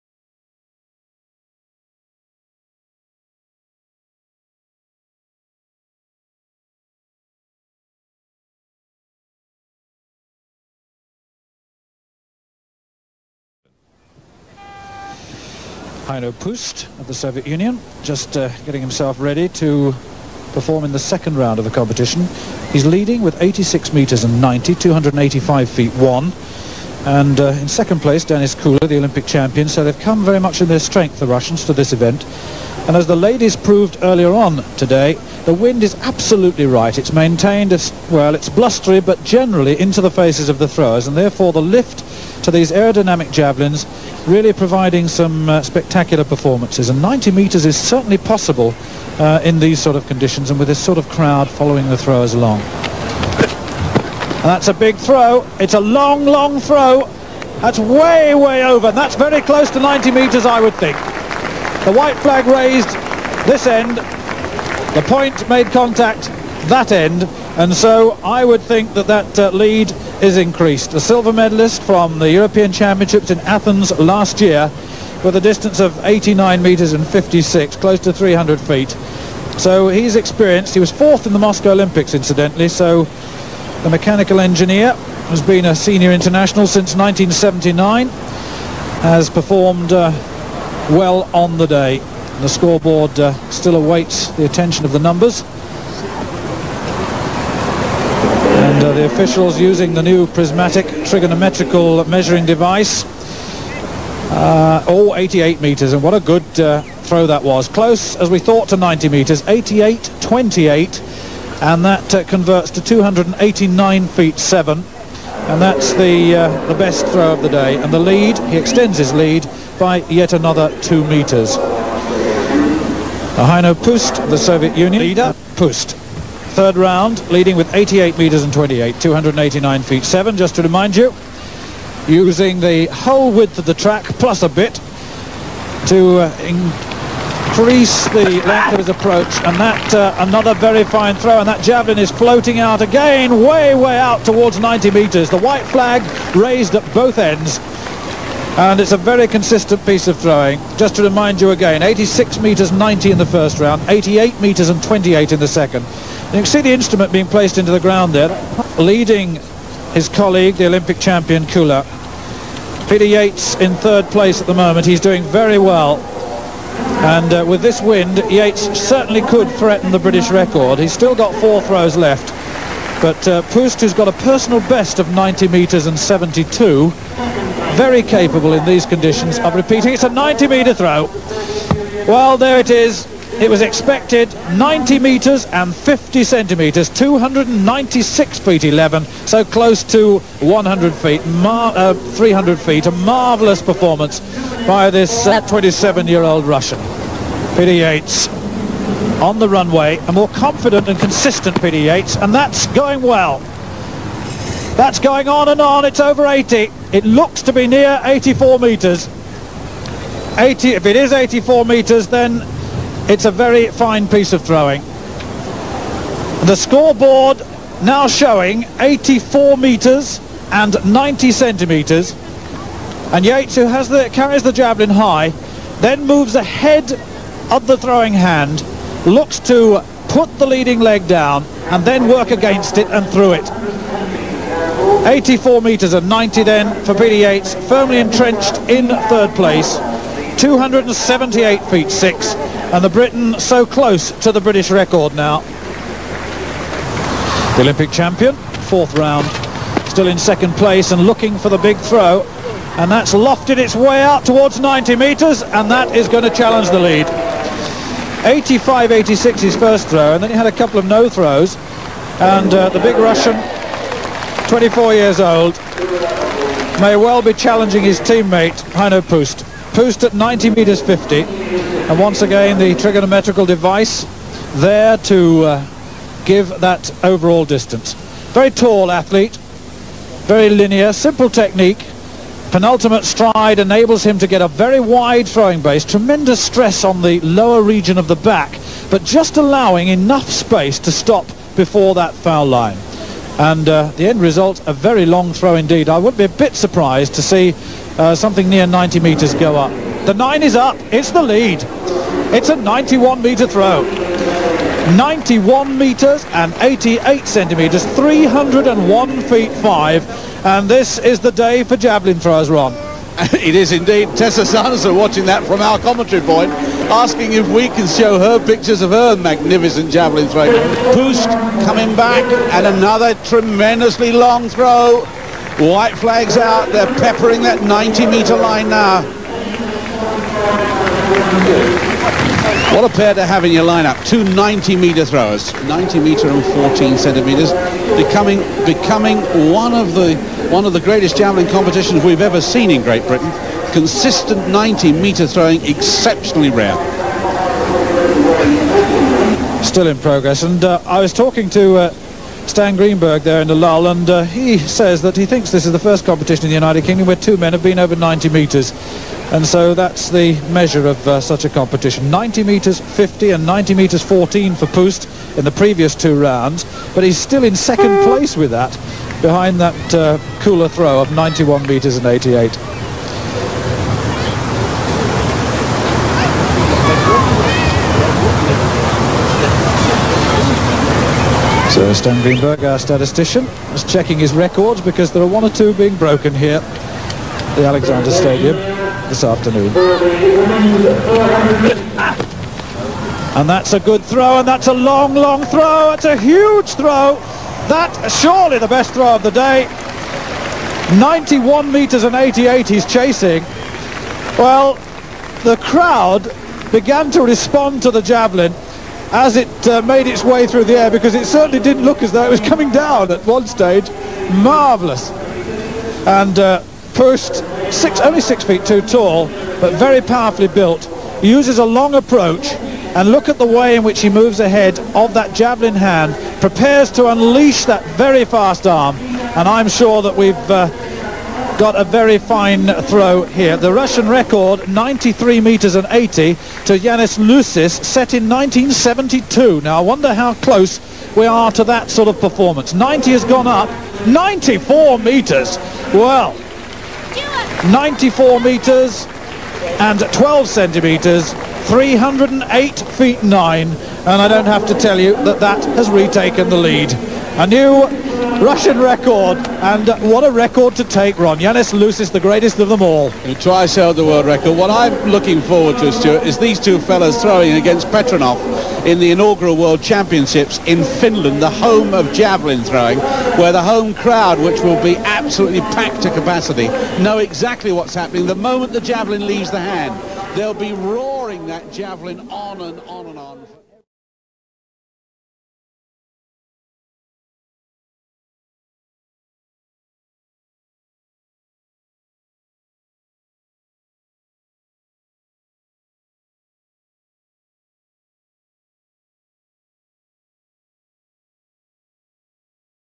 men javelin competition UK v. USSR Birmingham 1983-06-05
1. H Puuste (USSR) 94.20 NR
2. D Kula (USSR) 91.88
3. P Yates (UK) 84.90
4. S Glebov (USSR junior) 77.18
5. M Hill (UK junior) 74.16
6. M Humphries 73.34